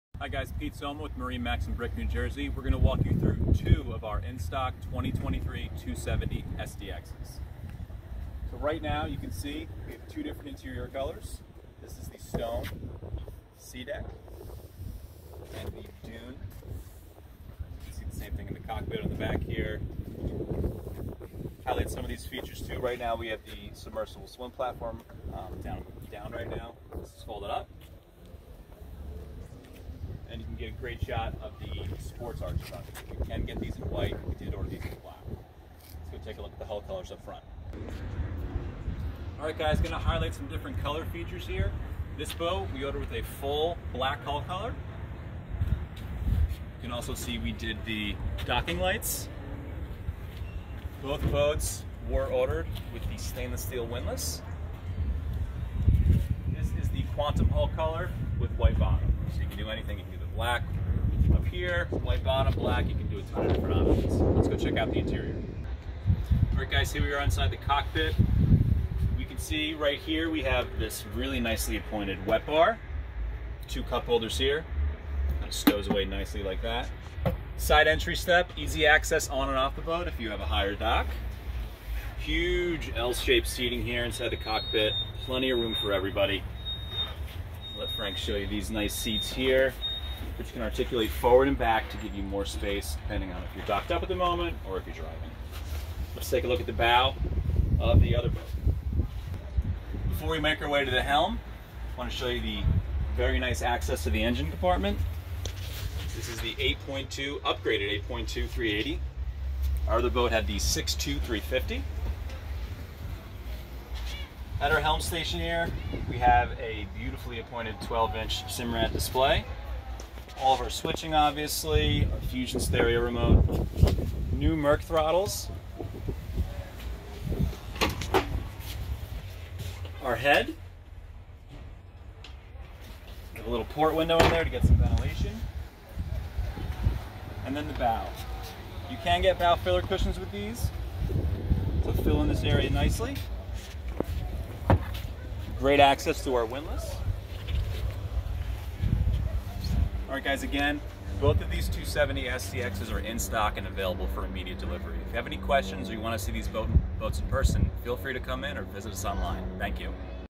2023 Sea Ray SDX 270 Boat For Sale at MarineMax Brick, NJ
Make a splash without rocking the boat: The SDX 270 is geared for incredible stability, comfort and handling. Sea Ray's purpose-driven engineering encompasses everything from a precision-molded hull right down to details like helm visors that minimize glare on gauges, and optional, soft, SeaDek swim platform matting underfoot. So for passengers of all ages looking to carve wake, catch up with loved ones or simply catch rays, the SDX 270 inspires lasting impressions.